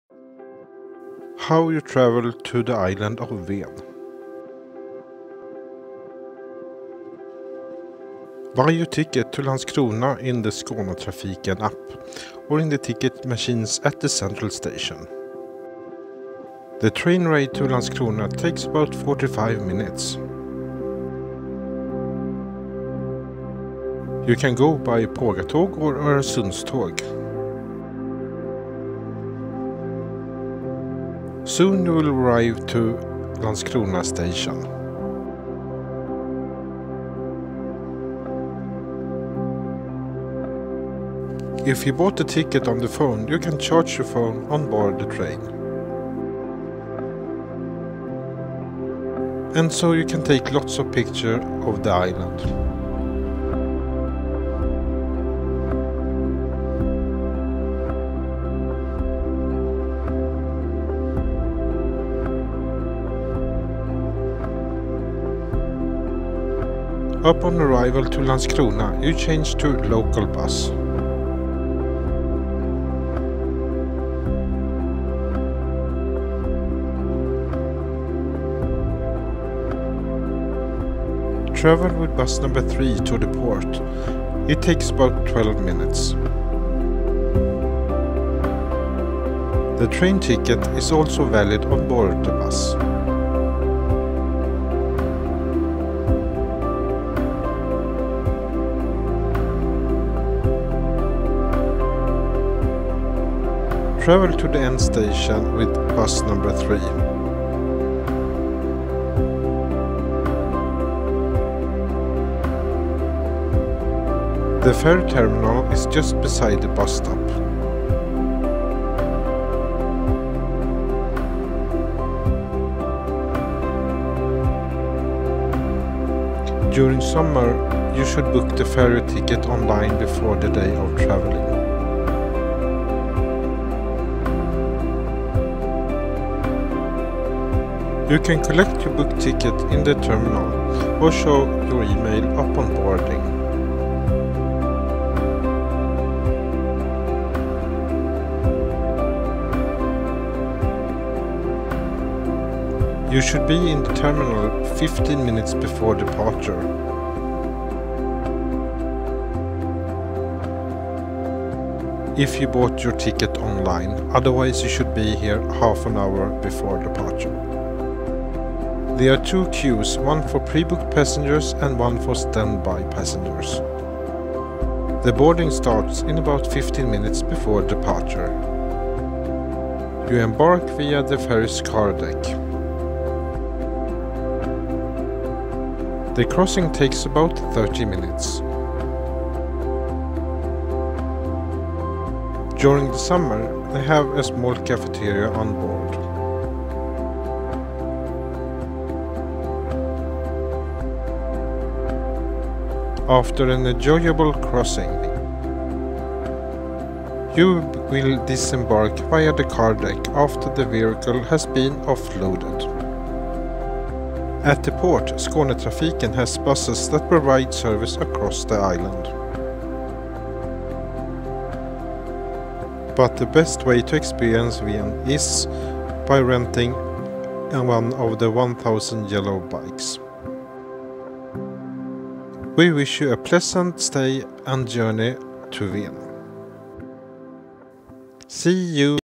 How to travel to the island Ven
In this video, we give you some tips on how you easy and environmentally friendly travel to the island of Ven. The island in the mittle of Öresund between Sweden and Denmark